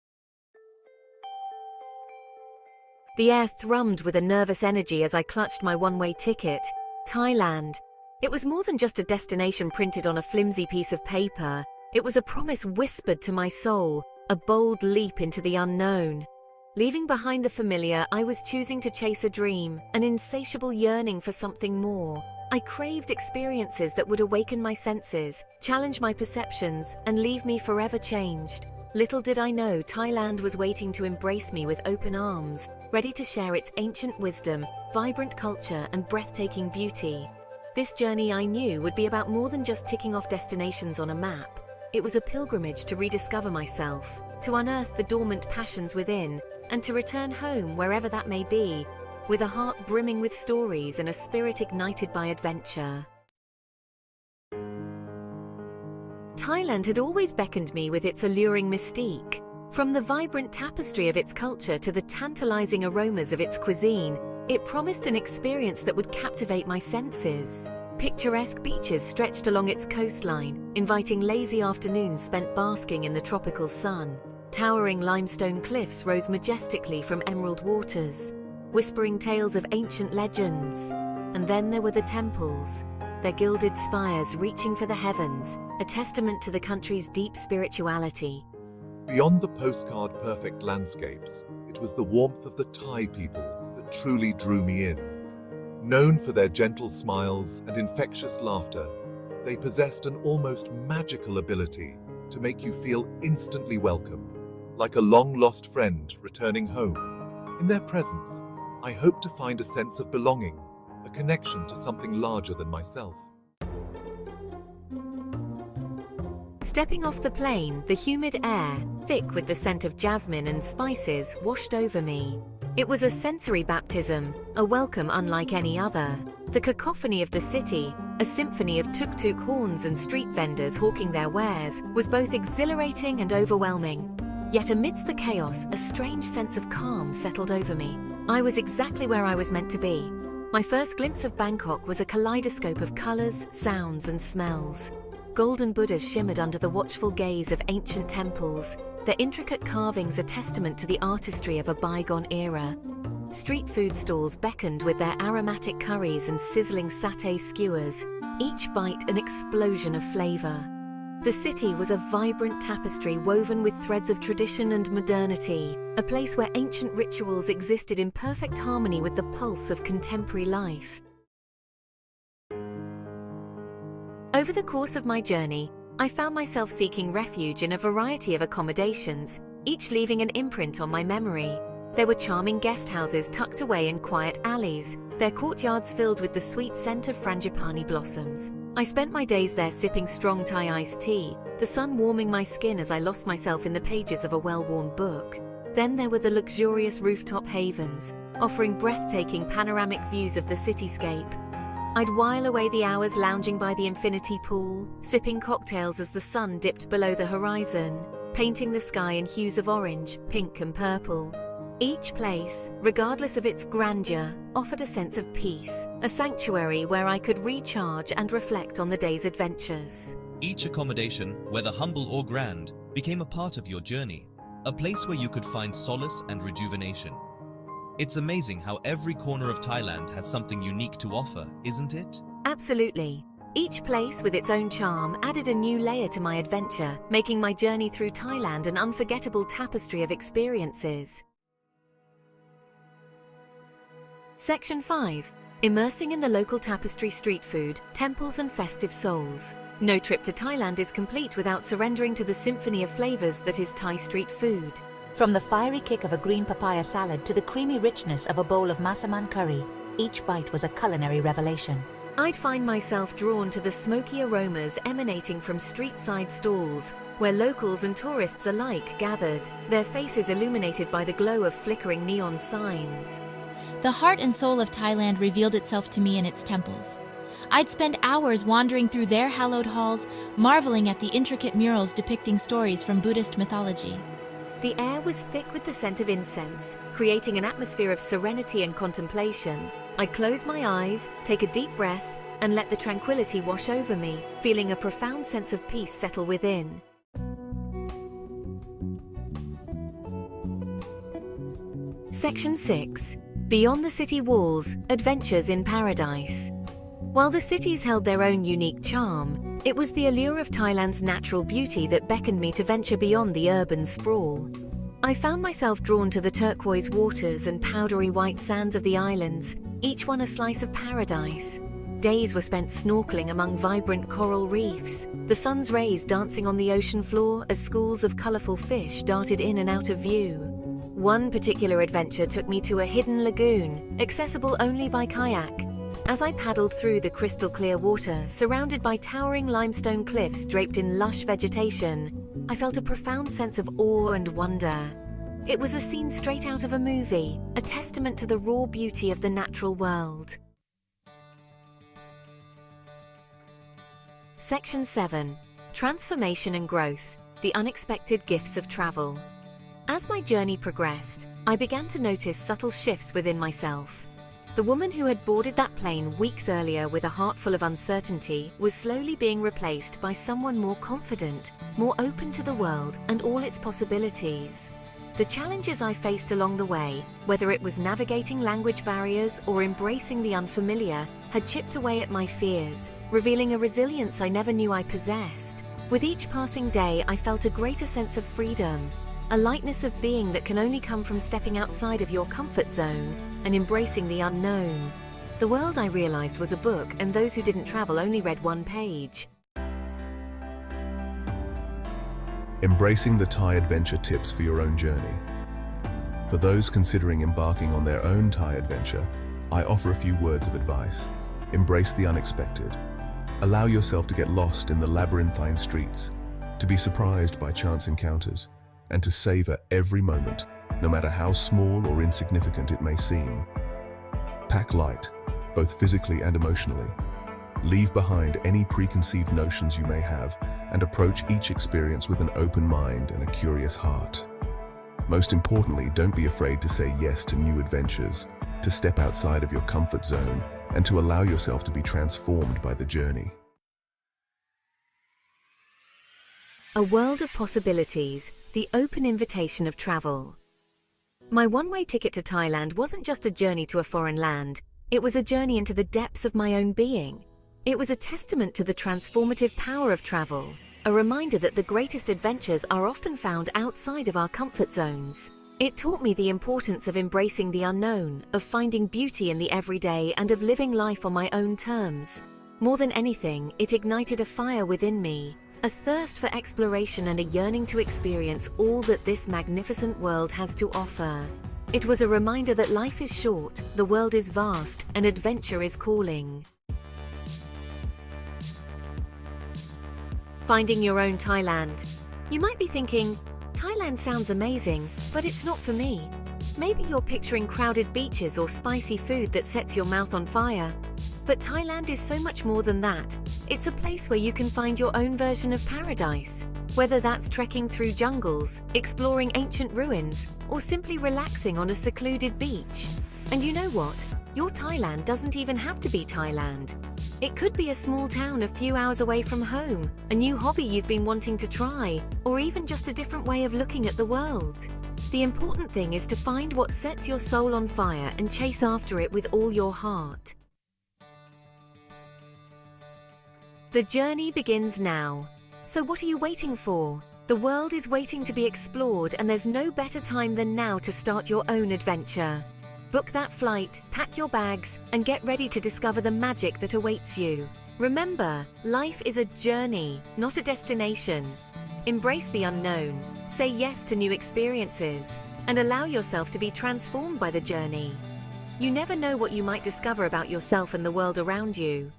Why a One-Way Ticket to Thailand Was My Best Decision Ever!
Embark on a virtual journey to Thailand and discover the ultimate hidden gem - a breathtaking rooftop pool that will leave you in awe. Join me as I reminisce about my one-way ticket to this tropical paradise and share why it was the best decision ever. From the stunning infinity pool to the luxurious amenities of this exotic getaway, this travel vlog will surely inspire your next adventure. Get ready to add this luxury resort experience to your bucket list as we explore the beauty of Asia and soak in the beauty of Thailand. Pack your bags and get ready for a virtual escape to this rooftop oasis!